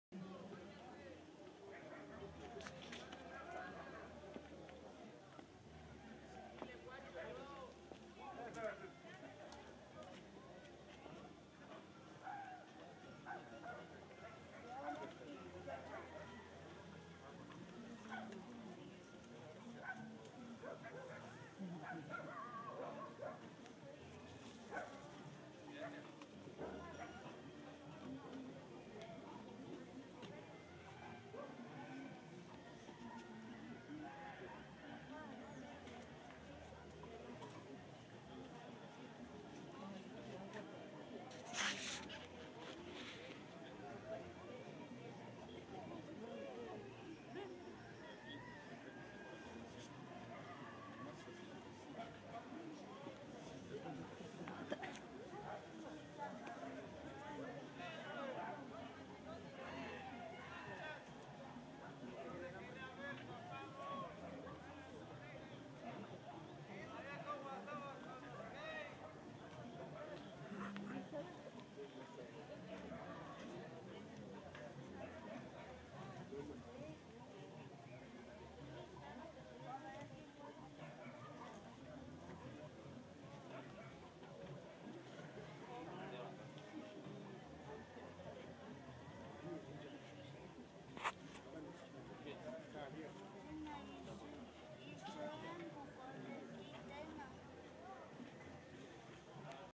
Dia De Muertos
En el panteón de Santiago Azajo, la gente se reúne con sus seres queridos desde muy temprano entre las 4:00 AM en adelante algunos un poquito mas temprano,